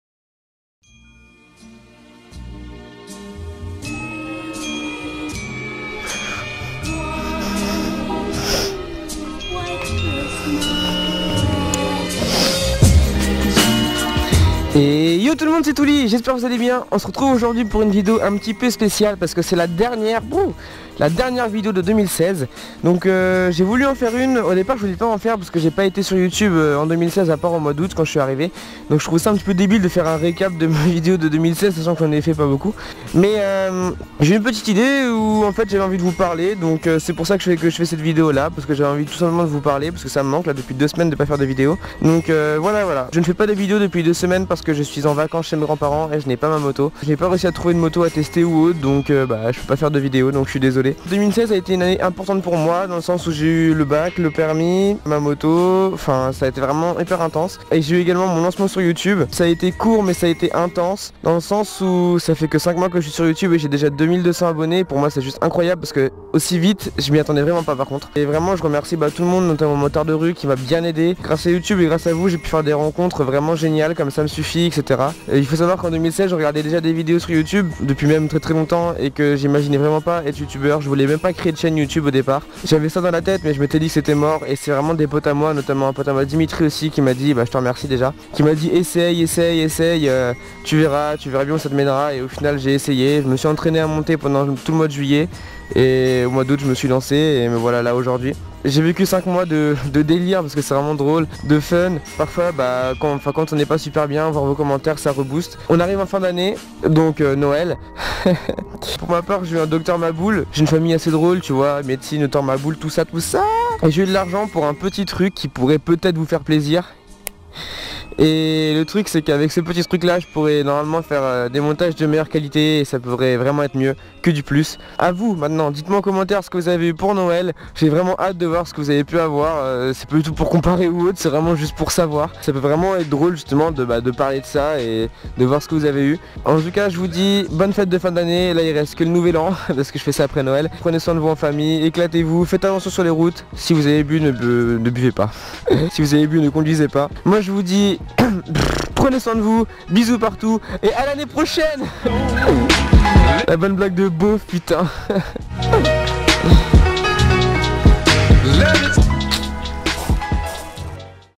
Mon Année 2016 !! 🎁☃🌲
Hey les biaaatchs ! Petite vidéo pour vous raconter ma vie mddr ! Merci pour cette fin d'année qui à été l'une des plus belles à mes yeux ! Ne changez rien car vous êtes en OR !
Merci à vous tous pour les 2247 abonnés !
Je prévois du Lourd & du changement pour 2017 !
Dites moi en commentaire ce que vous avez eu Pour Noël !
Prennez soin de vous & bisous partout !

MA MOTO : YAMAHA MT 125 RACE BLUE 2016
GO PRO HERO 4 SILVER
MICRO BOYA POUR GO PRO

TWEALY.